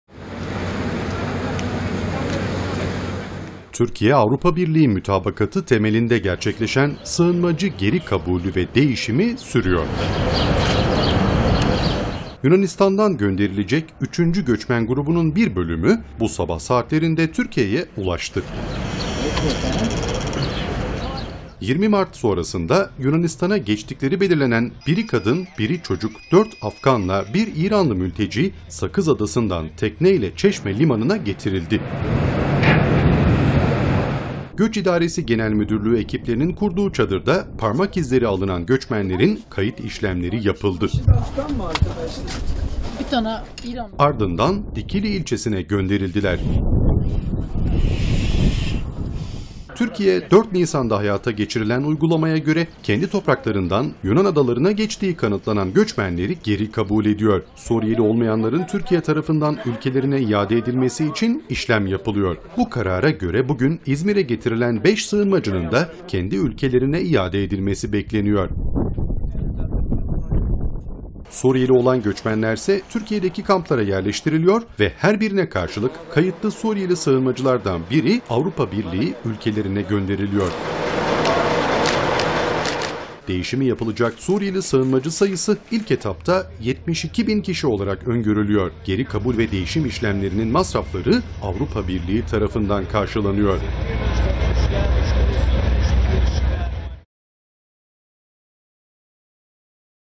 Geri Kabul Sürüyor
Avrupa Birliği’yle yapılan geri kabul anlaşması kapsamında, mülteciler gruplar halinde Türkiye’ye gönderiliyor. Üçüncü göçmen grubu, bugün Yunanistan'dan Çeşme Limanı'na getirildi.